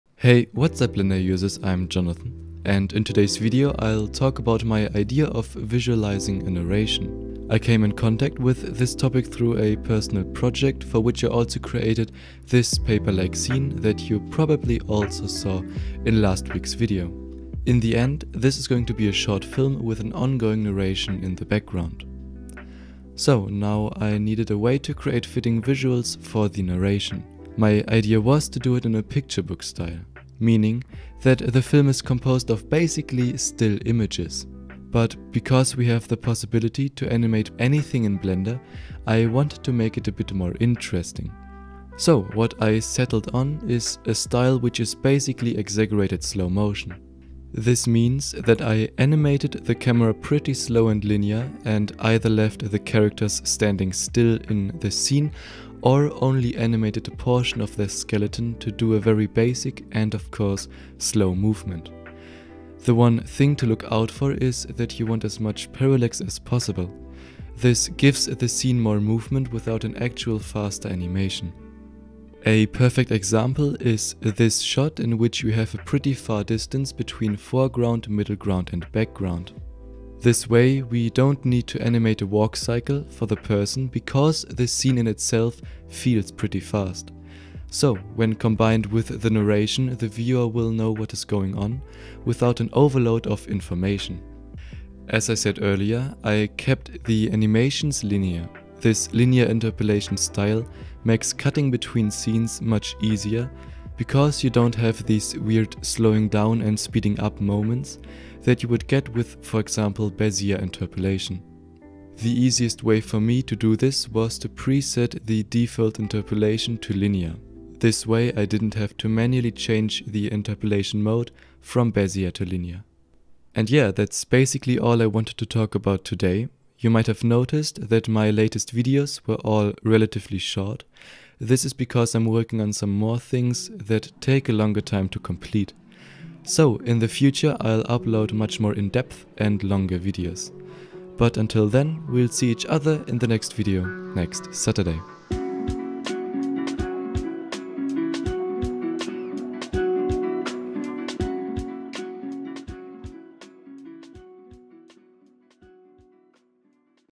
Visualize a Narration [Blender 2.9] | English
In this tutorial, I show you how to visualize a narration in Blender.

**Partners**